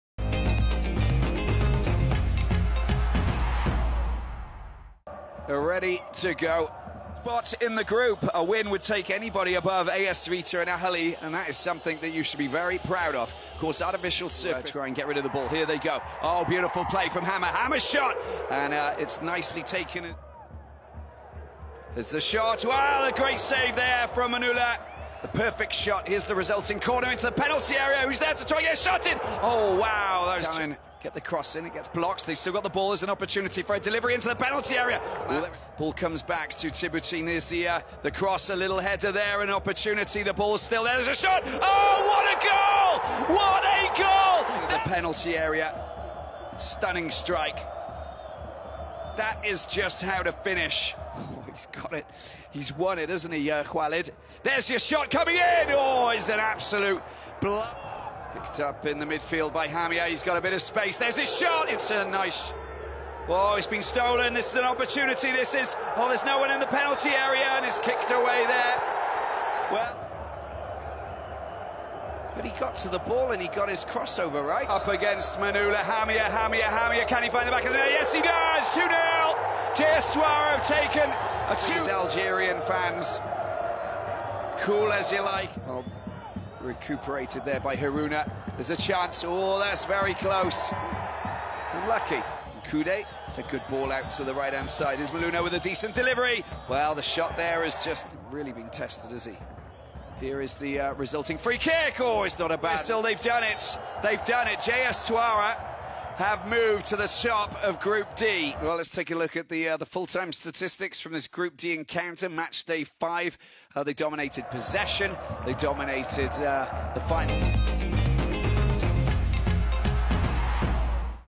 ملخص مباراة شبيبة الساورة الجزائري 2 - 0 سيمبا التنزاني الجولة الخامسة JSS VS SSC
دوري أبطال إفريقيا 2019
الجولة الخامسة
الجولة 5
09/03/2019